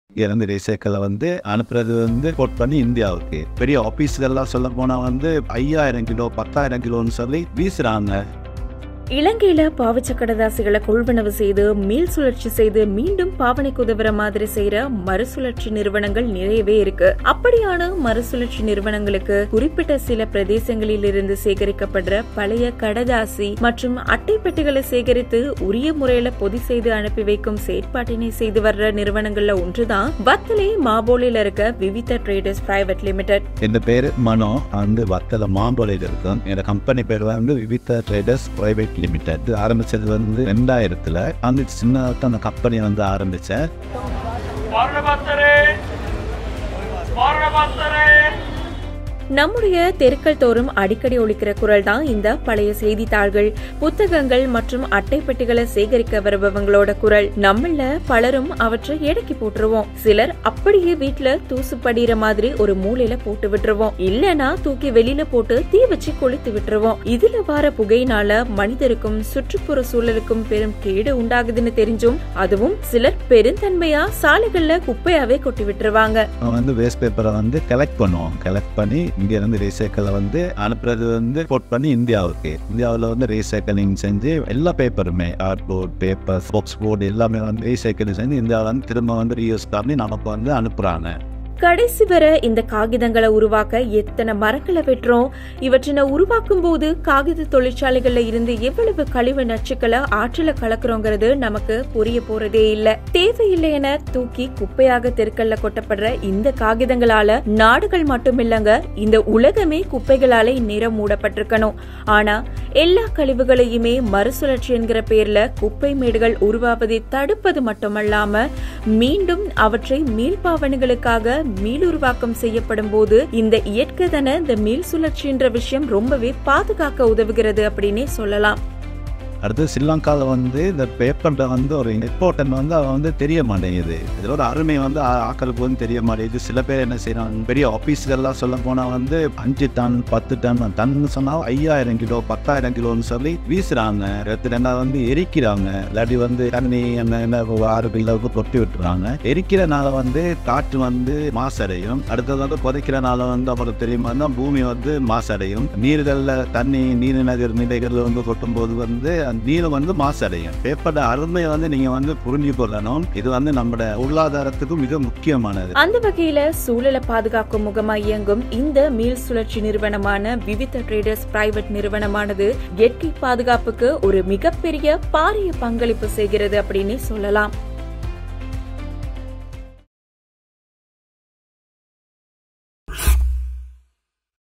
மீள்சுழற்சி செய்யப்படும் பழைய பத்திரிகைகள் | Recycled old magazines | நாடி | Nadi.lk
பழைய பத்திரிகைகளை எடைக்கு  போட்டுவிடுவோம், சிலர் அப்படியே வீட்டில் தூசு படிய  ஒரு மூலையில்  போட்டு விடுவோம் இல்லையேல் தூக்கி வெளியில் போட்டு தீ வைத்து கொளுத்தி விடுவோம் இதனால் எந்த அளவு சூழல் மாசடைகின்றது என்பதுபற்றி நாம் அனைவரும் அறிந்த விடயமே.

ஆனால் சிறிது நேரம் இதற்காக ஒதுக்கி இந்த காணொளியை பார்த்தால் உங்கள் வீட்டிலுள்ள பழைய காகிதங்களை இயற்கைக்கு தீங்கு அளிக்காத முறையில் மாற்ற கூடியதற்கான ஆலோசனை கிடைக்கும்.

வாழ்க்கை வாழ்வதற்கே!